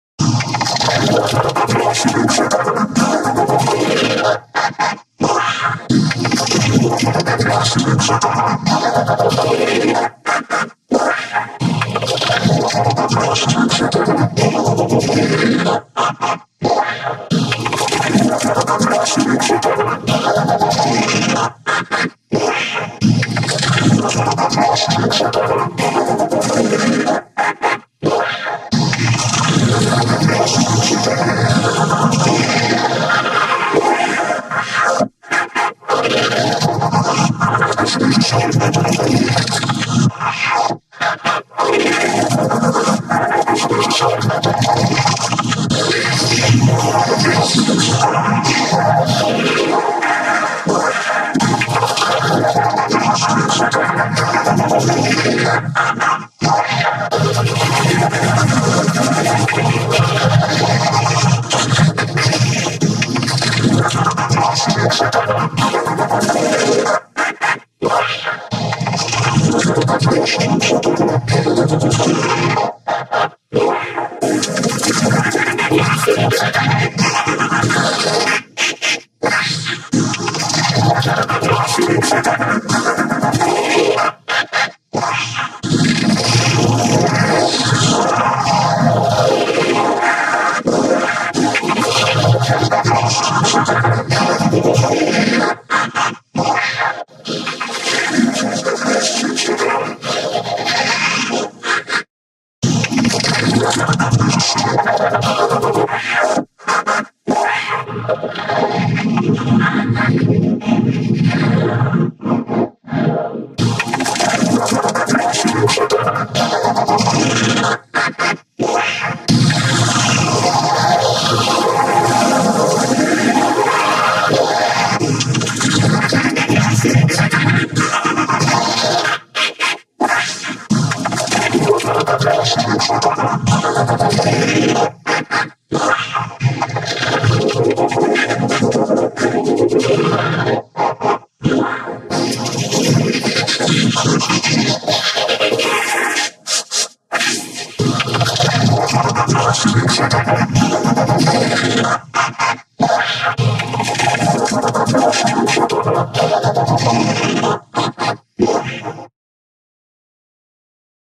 (REQUESTED) Klasky Csupo 1998 Super Effects in Phased Effect 4.0 (FIXED)
Fixed for Айгуль Есенжолова because he used MegaPhoto, and I don't use MegaPhoto.